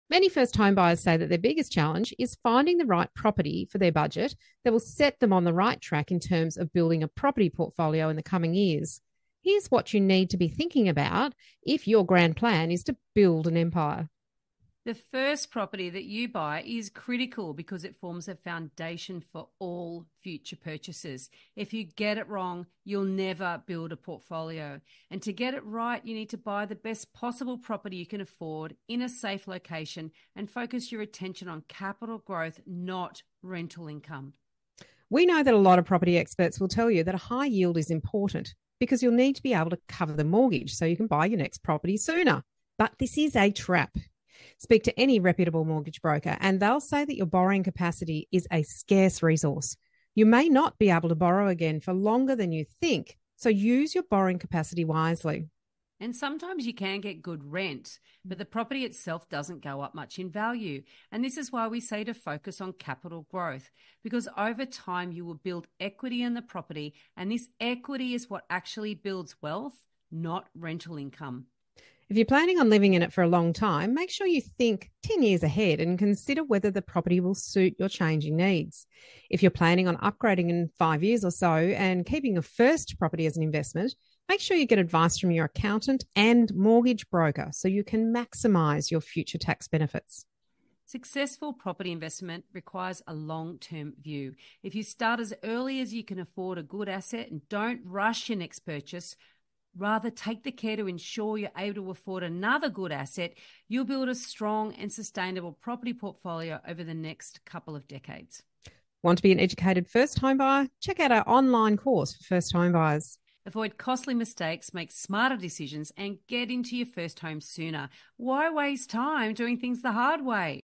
How do you build a property portfolio for beginners
Many first home buyers say that their biggest challenge is finding the right property for their budget that will set them up on the right track in terms of building a property portfolio in the coming years. Here’s what you need to be thinking about if your grand plan is to build an empire.

The first property you buy is critical because it forms the foundation for all future purchases. If you get it wrong, you'll never build a portfolio. To get it right you need to buy the best possible property you can afford, in a safe location and focus your attention on capital growth NOT rental income.

We know that a lot of property experts will tell you that a high yield is important because you’ll need to be able to cover the mortgage so you can buy your next property asap, BUT, this is a trap. Speak to any reputable mortgage broker and they’ll say that your borrowing capacity is a scarce resource - you may not be able to borrow again for longer than you think - so use your borrowing capacity wisely.

And sometimes you can get good rent, but the property itself doesn’t go up much in value. This is why we say focus on capital growth. Over time you will build equity in the property and this equity is what actually builds wealth, not rental income.

If you are planning on living in it for a long time, make sure that you think 10 years ahead and consider whether the property will suit your changing needs. If you are planning on upgrading in 5 or so years and keeping your first property as an investment, make sure you get advice from your accountant and mortgage broker so you can maximise future tax benefits.

Successful property investment requires a long term view. If you start as early as you can afford a good asset and don’t rush your next purchase, rather take care to ensure you’re able to afford another good asset, you’ll build a strong and sustainable property portfolio over the next couple of decades.

Avoid costly mistakes, make smarter decisions, and get into your first home sooner. Why waste time doing things the hard way?